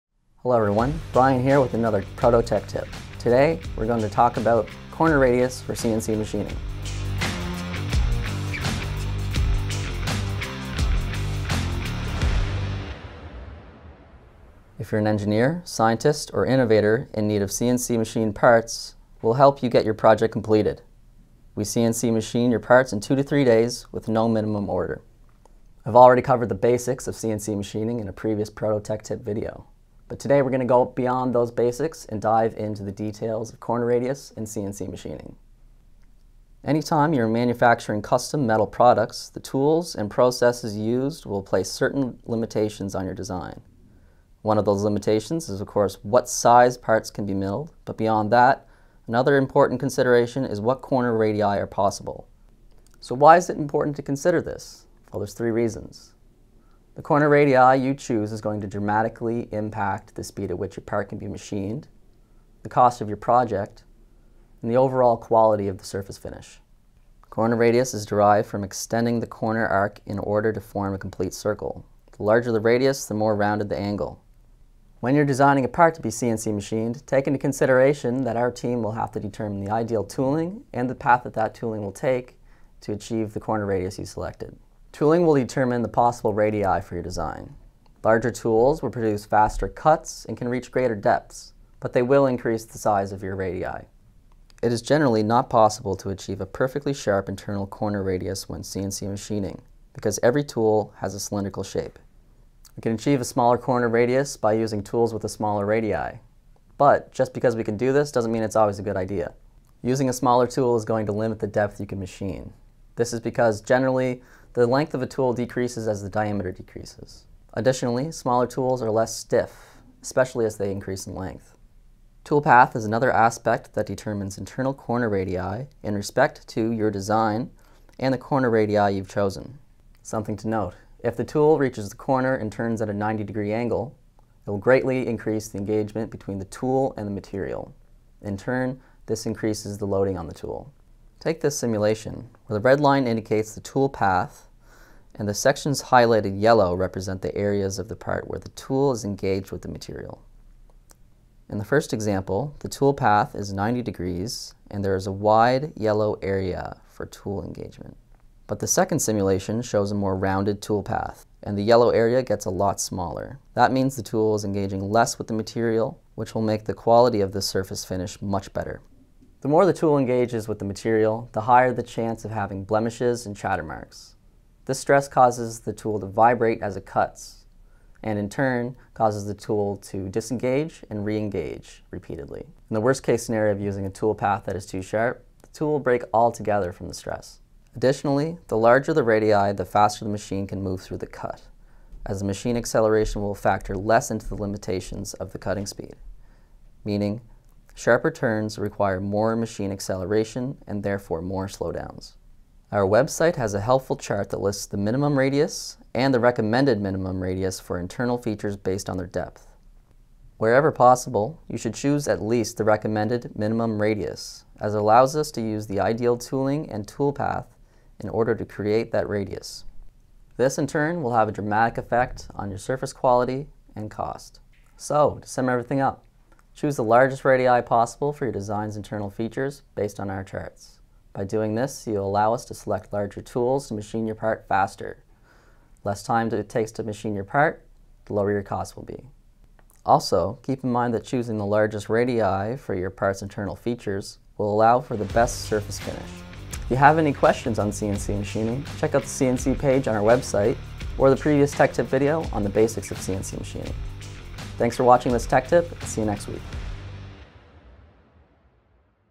Proto Tech Tip - CNC Corner Radius
This week, Brian from our Engineering & Design Services team goes beyond the basics to give you a more in-depth look at an important concept of CNC Machining; corner radius. Its important to note when designing your CNC parts and enclosures, the corner radii you choose can have a dramatic impact on the speed, cost and quality of your finished design.

Check out our web pages dedicated to CNC Milling and CNC Turning

Check out our blog article on how corner radius affects CNC Machining